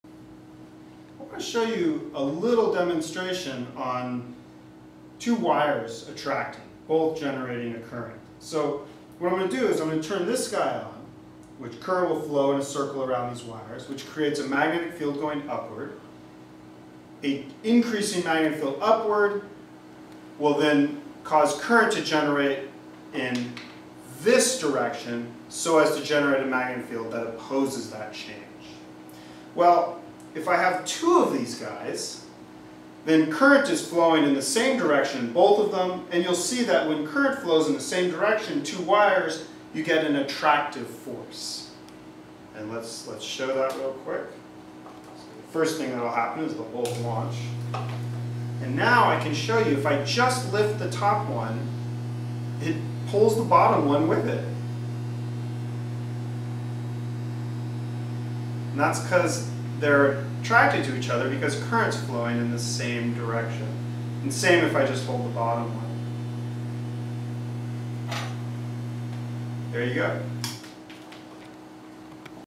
Ring Launcher two wires attracting
The attraction of two wires carrying current in the same direction is demonstrated using a ring launcher.